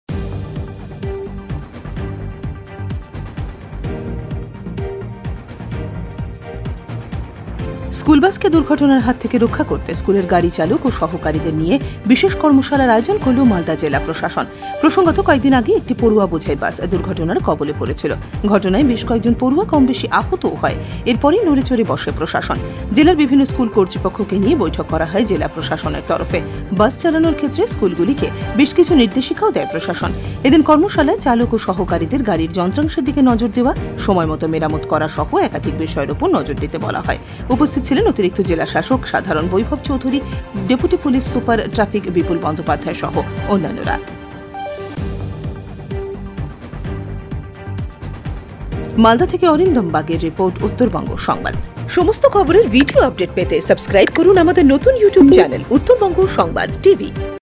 দুর্ঘটনা রুখতে স্কুলবাস চালকদের নিয়ে কর্মশালা
স্কুলবাসকে দুর্ঘটনার হাত থেকে রক্ষা করতে স্কুলের গাড়িচালক ও সহকারীদের নিয়ে বিশেষ কর্মশালার আয়োজন করল মালদা জেলা প্রশাসন।